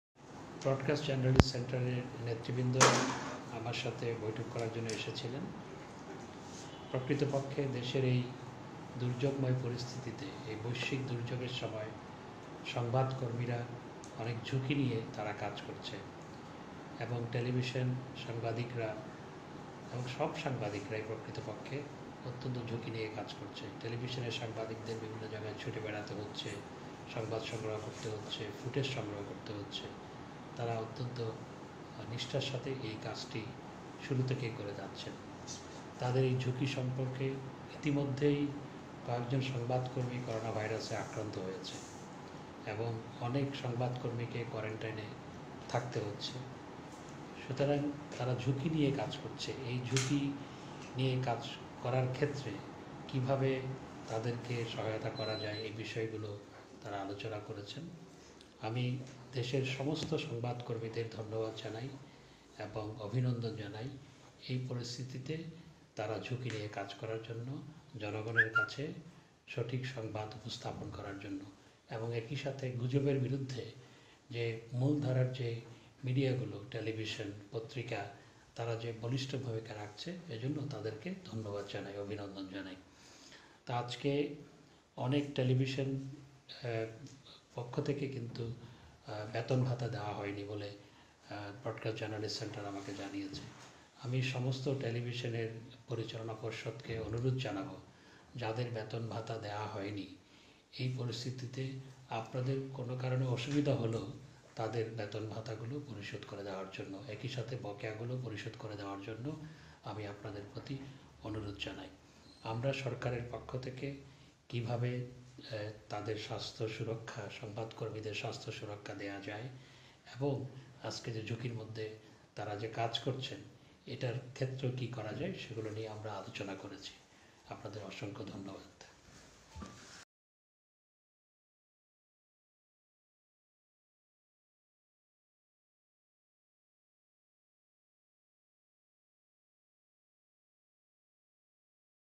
টিভি কর্মীদের স্বাস্থ্যসুরক্ষা নিশ্চিত করার আহ্বান
টেলিভিশন চ্যানেলের সাংবাদিক ও কর্মীদের স্বাস্থ্যসুরক্ষা নিশ্চিত করা ও বকেয়াসহ বেতন-ভাতাদি পরিশোধের জন্য মালিকদের প্রতি আহবান জানিয়েছেন তথ্যমন্ত্রী ড. হাছান মাহমুদ। শনিবার দুপুরে ঢাকায় মিন্টু রোডের সরকারি বাসভবনে ব্রডকাস্ট জার্নালিস্ট সেন্টার নেতৃবৃন্দের সাথে বৈঠকের পর এ আহবান জানান তিনি।